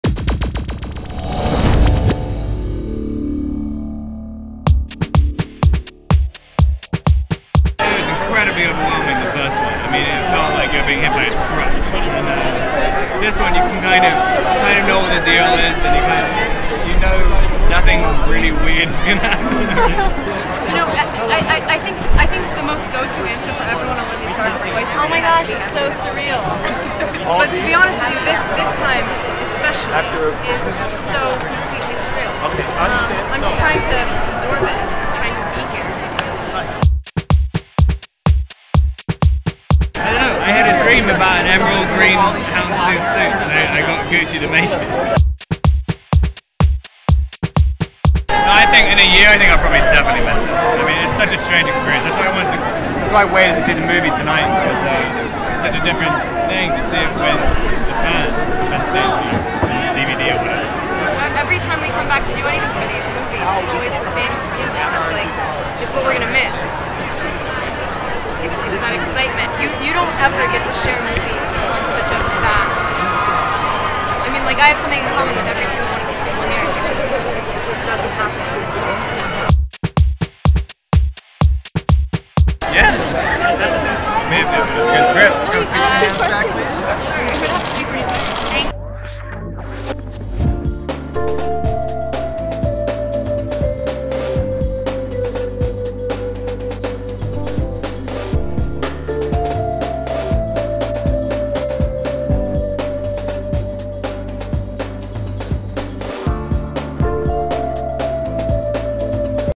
Kristen Stewart + Robert Pattinson at the "Breaking Dawn Part 2" premiere
Kristen Stewart and Robert Pattinson hit the black carpet for the world premiere of "the Twilight Saga: Breaking Dawn — Part 2" in Los Angeles.